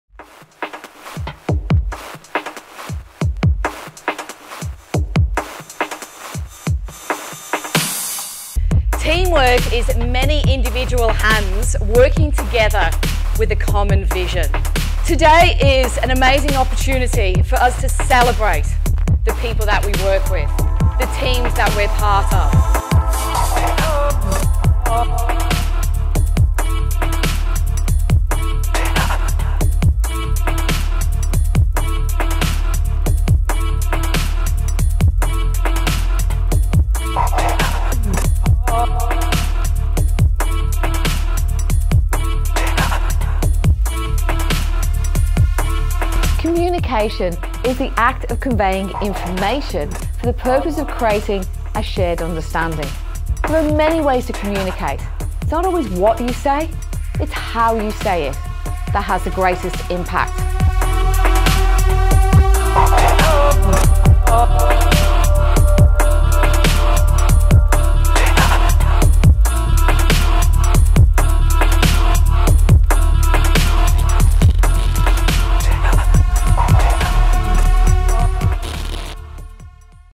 Begin, Believe, Become - Get to know Tracey Hughes, Professional Speaker - Hairdressing Education
Discover new training methods with guaranteed results and motivation to make your dreams come true.

Tracey Hughes - Professional Speaker.
Innovative Creativity, Business Solutions for the Hairdressing Industry!